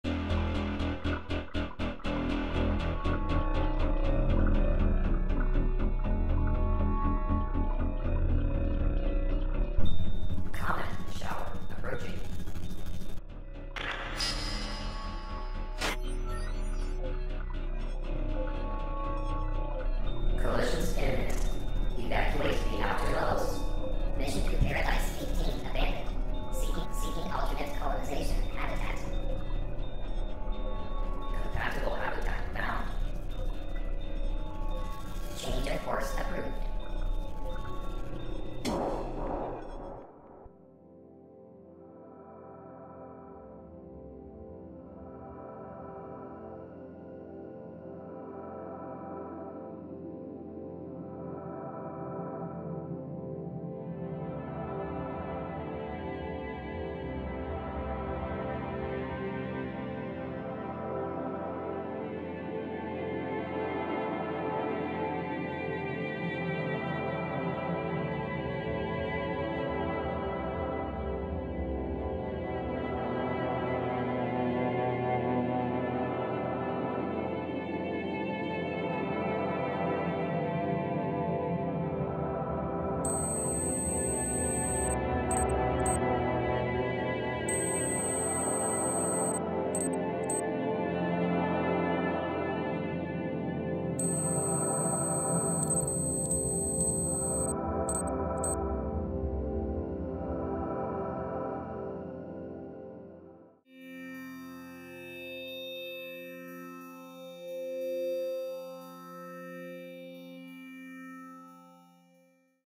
Book Trailer: Tundra 37
Book 2 in The New Dawn Series

Gemme is a hi-tech matchmaker who pairs the next generation of Lifers aboard the Expedition, a deep space transport vessel destined for Paradise 18. When the identity of her lifemate pops up on her screen, she's shocked that he's the achingly gorgeous and highly sought after Lieutenant Miles Brentwood—a man oblivious to her existence. Believing everyone will think she contrived the match, she erases it from the computer's memory.

Just as comets pummel the ship and destroy the pairing system forever.

With the Expedition disabled, the colonists must crash land on the barren ice world of Tundra 37 where Gemme is reassigned to an exploratory mission, led by Lieutenant Brentwood. Only in the frozen tundra does she understand the shape of his heart and why the computer has entwined their destinies.